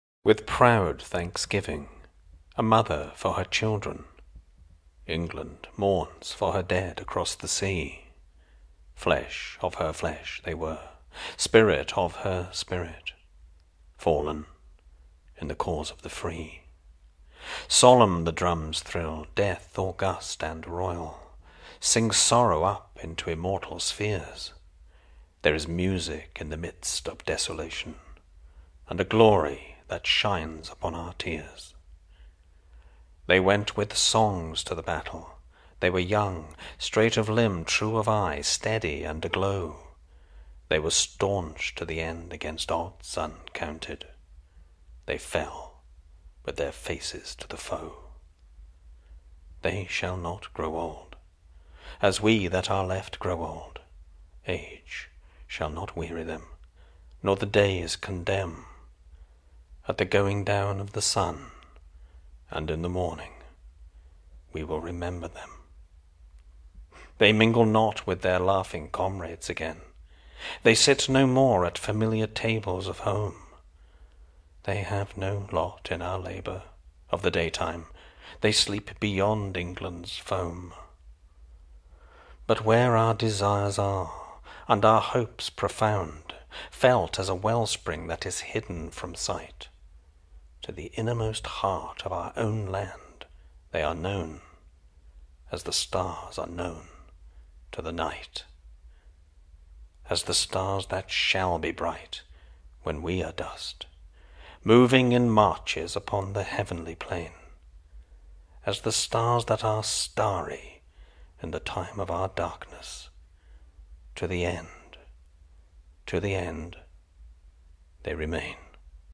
'For The Fallen' by the English poet, Laurence Binyon 1869 - 1943
This famous poem memorialises the fallen of The Great War and contains the verse:
'They shall grow not old, as we that are left grow old:
Age shall not weary them, nor the years condemn.